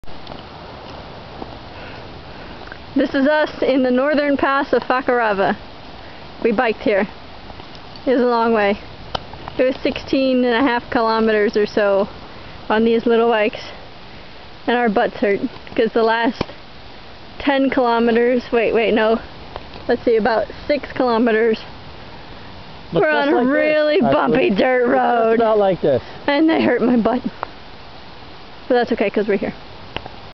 Fakarava North Point beach biking
We're in Fakarava, in the Tuamotu of French Polynesia!
Follow us as we travel the world!